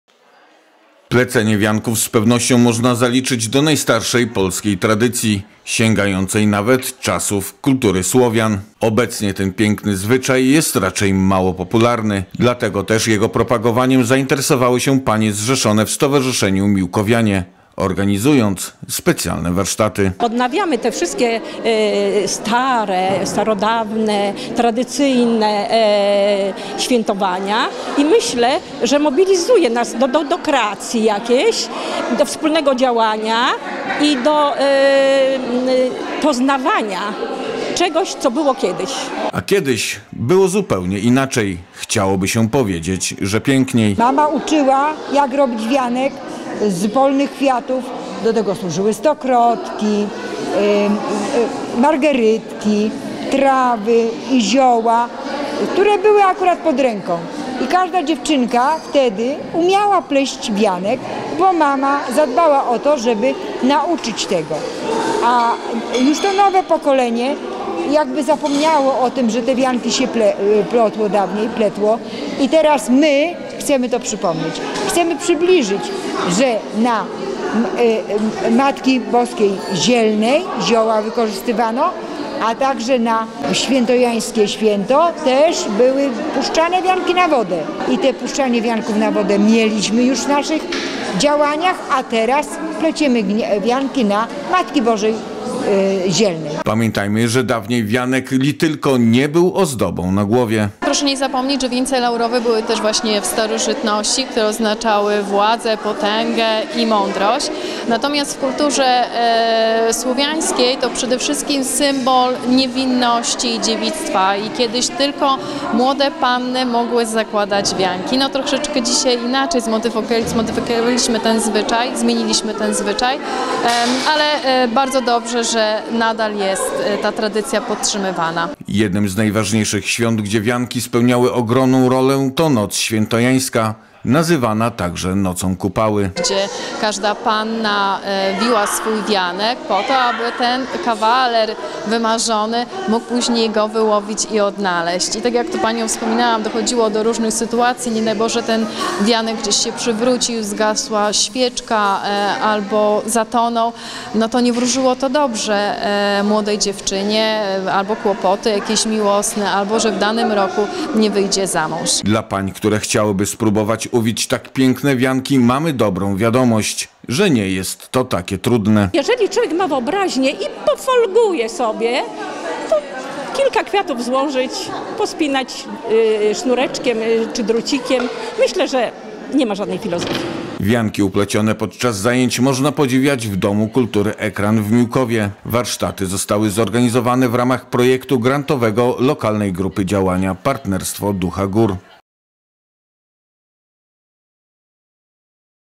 W Miłkowie podtrzymują tradycję plecenia wianków
Plecenie wianków z pewnością można zaliczyć do najstarszej polskiej tradycji sięgającej czasów kultury Słowian. W Domu Kultury Ekran w Miłkowie w ramach projektu grantowego Lokalnej Grupy Działania Partnerstwo Ducha Gór odbyły się warsztaty ich tworzenia.